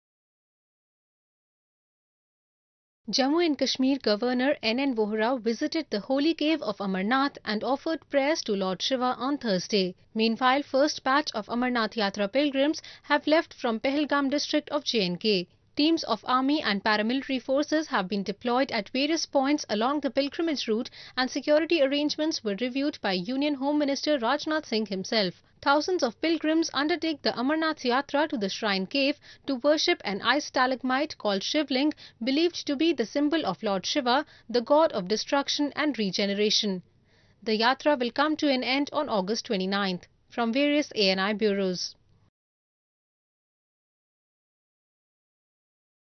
J&K governor NN Vohra visits holy cave of Amarnath
Amarnath/ Chandanwari, July 02 (ANI): Jammu and Kashmir Governor NN Vohra visited the holy cave of Amarnath and offered prayers to Lord Shiva on Thursday. Meanwhile, first batch of Amarnath Yatra pilgrims have left from Pahalgam district of J&K. Teams of army and paramilitary forces have been deployed at various points along the pilgrimage route and security arrangements were reviewed by Union Home Minister Rajnath Singh. Thousands of pilgrims undertake the Amarnath Yatra to the shrine cave to worship an ice stalagmite called Shivlinga, believed to be the symbol of Lord Shiva, the god of destruction and regeneration. The yatra will come to an end on August 29.